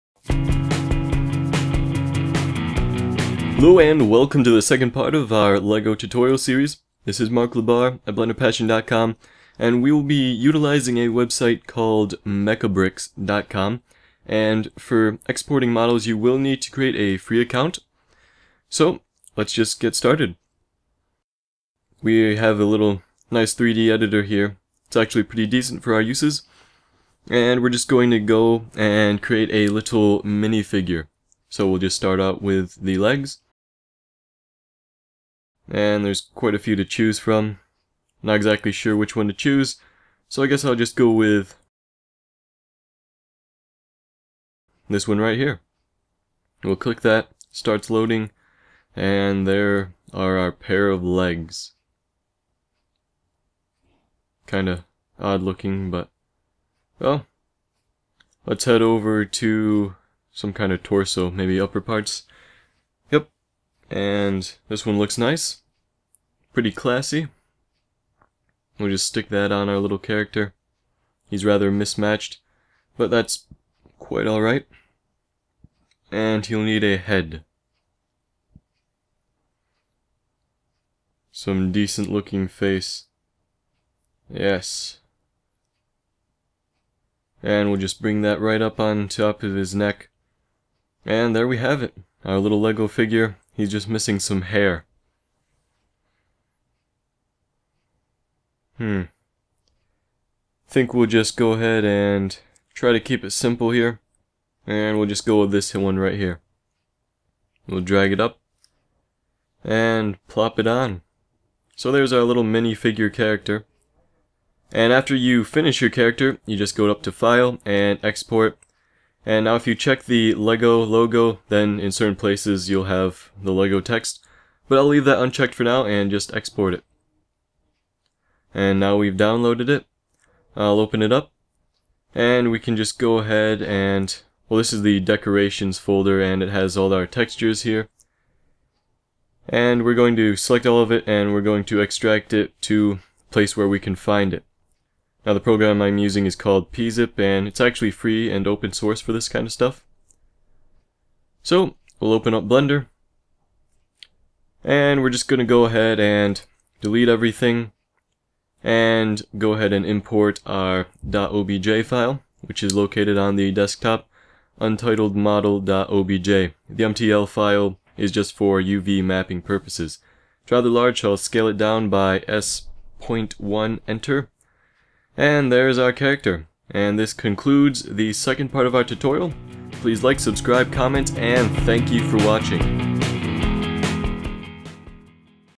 Learn How to Create 3D Lego Scenes - Part 2: Online Resources
Using to quickly create a Lego figure to import into Blender.